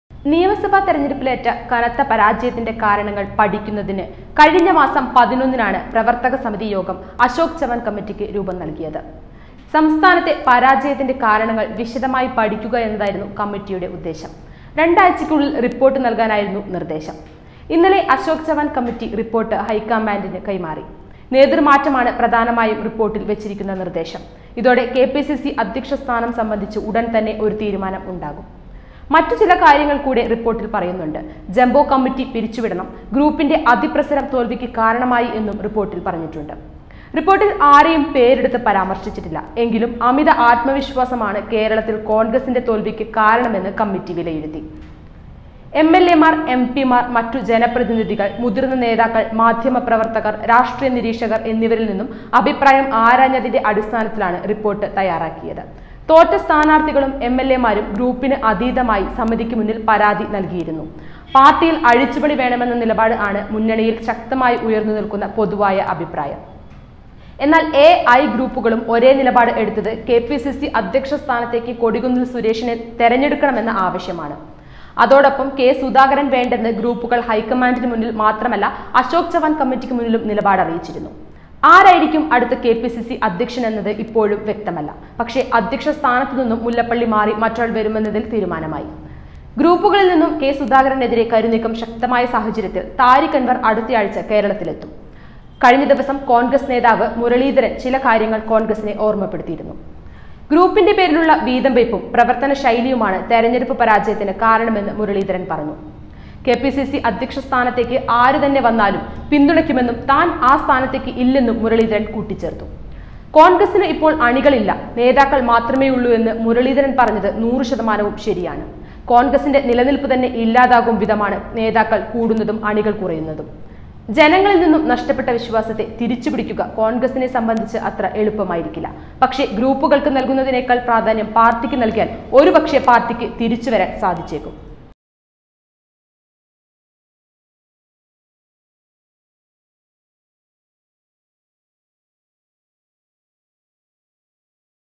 ആരായിരിക്കും അടുത്ത കെ പി സി സി അധ്യക്ഷൻ ? II UDF II MULLAPPALLY RAMACHANDRAN II CONGRESS
അശോക് ചവാന്‍ കമ്മിറ്റി റിപ്പോര്‍ട്ട് ഹൈക്കമാന്‍ഡിന് കൈമാറി. നേതൃമാറ്റമാണ് പ്രധാനമായും റിപ്പോര്‍ട്ടില്‍ വെച്ചിരിക്കുന്ന നിര്‍ദേശം. ഇതോടെ കെപിസിസി അധ്യക്ഷ സ്ഥാനം സംബന്ധിച്ച് ഉടന്‍ തന്നെ ഒരു തീരുമാനം ഉണ്ടാവും